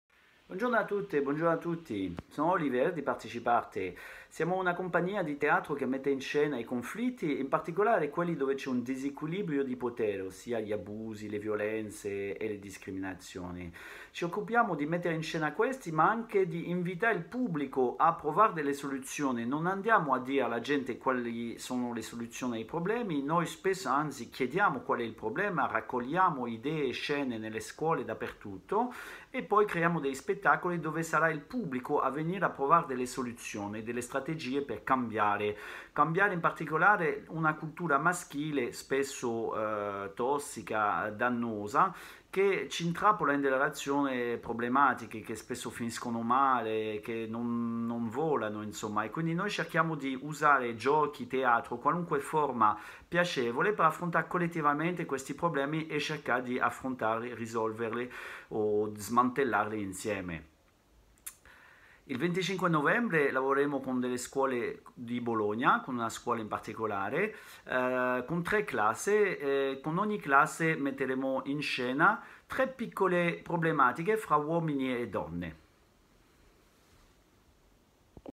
Noino.org: Teatro Forum contro la violenza di genere - Olivier Malcor
Mercoledì 25 novembre, in occasione della Giornata internazionale per l’eliminazione della violenza di genere, proponiamo una serie di incontri in digitale con due classi dell'Istituto tecnico industriale Aldini Valeriani di Bologna.

L’iniziativa si inserisce all’interno del progetto Noino.org della Fondazione del Monte, coordinato dall’associazione Il progetto Alice e gestito da Rete Attraverso lo Specchio. Noino.org ha l’obiettivo di contrastare la violenza di genere partendo dal cambiamento dei modelli maschili e dalla sensibilizzazione dei più giovani attraverso percorsi educativi per le scuole superiori, corsi di formazione per insegnanti ed eventi pubblici per la comunità.

Per poter coinvolgere gli studenti in una riflessione sulla violenza di genere nel rispetto delle misure di contenimento del Covid-19, Noino.org approda al digitale con attività laboratoriali e teatrali a cura della compagnia PartecipArte.

L'attrice Claudia Signoretti e l'attore Olivier Malcor proporranno a due classi dell’Istituto tecnico industriale Aldini Valeriani di Bologna laboratori online per affrontare diverse problematiche legate alla cultura maschilista, ma anche alcuni sketch tratti da Amore mio - Quando una relazione diventa pericolosa, uno spettacolo di Teatro Forum sulla violenza maschile costruito per mostrare le tappe che portano alla violenza e suggerire soluzioni per aiutare chi si trova in una relazione pericolosa. Il teatro forum è un genere di spettacolo originale, in cui il pubblico può interrompere intervenendo per cambiare la storia, scoprendo così in presa diretta le difficoltà d’intervento, comprendendo strategie pericolose e individuando possibili vie d’uscita. In video Olivier Malcor ci spiega la strategia.